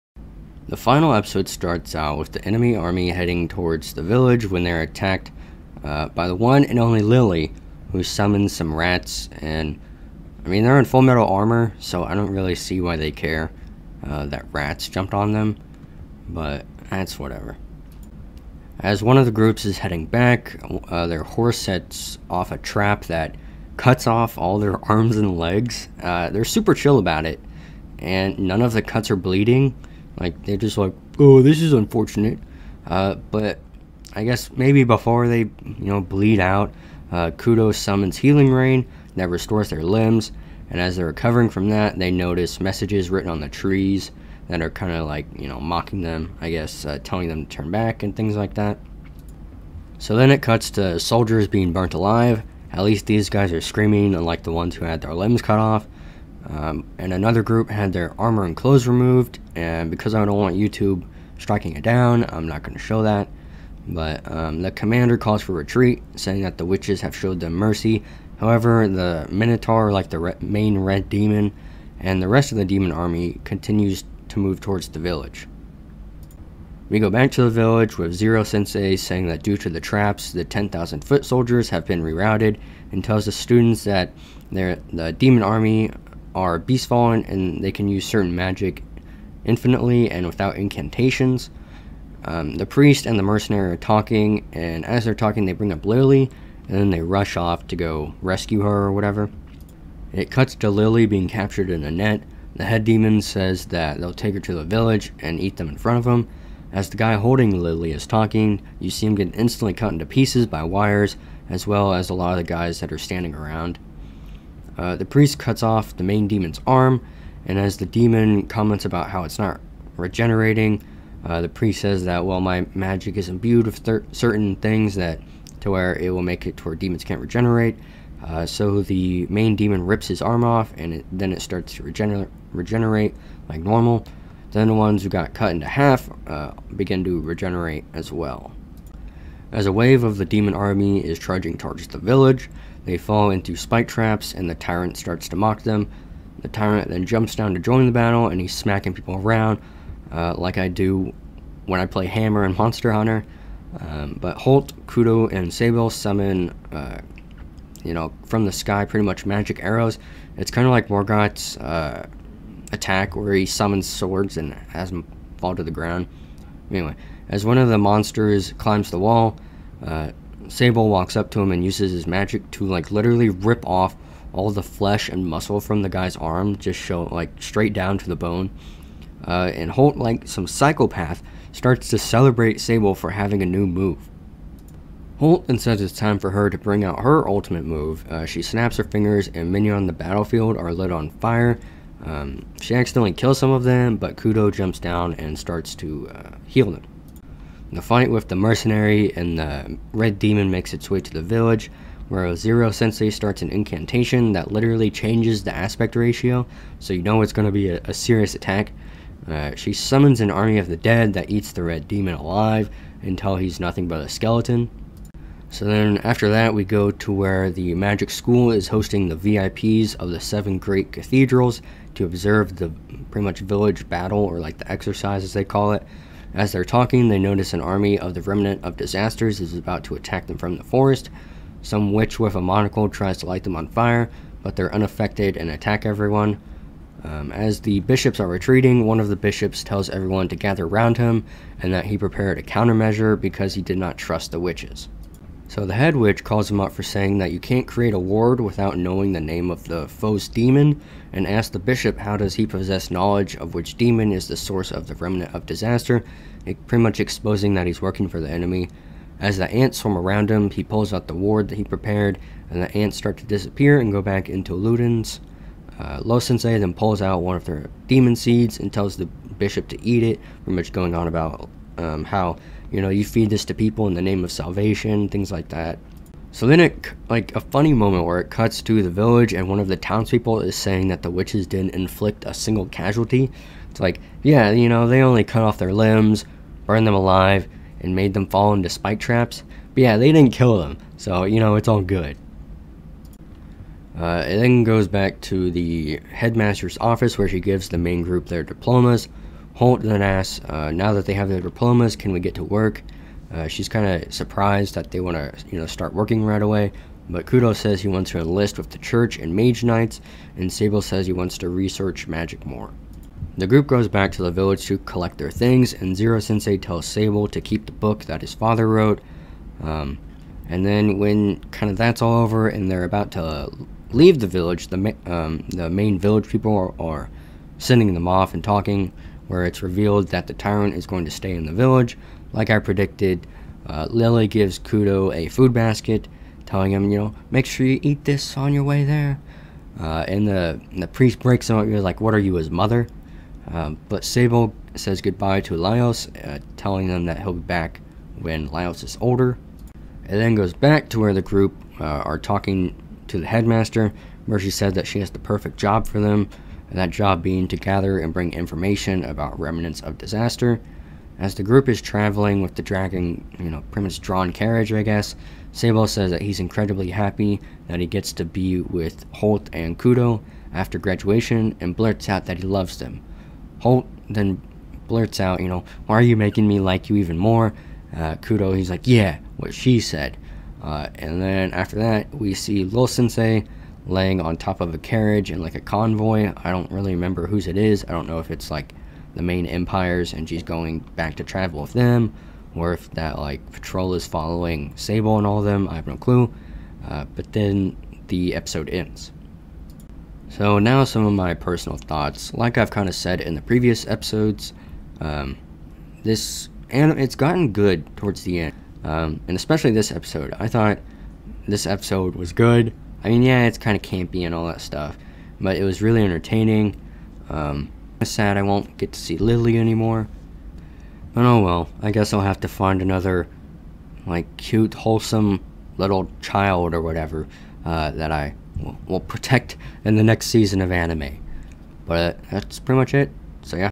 How will I live without Lily - Dawn of the Witch Ep12
The anime kept getting better and better, and this episode stays true to that. It wrapped up everything well, and while I doubt it will get a second season. If it does I will definitely watch it.

I watch all my anime during the weekend, and now that spring anime is over. I don't have anything planned until next week.

There are a couple of Manhwa I want to talk about, so I may make a video on those for this week.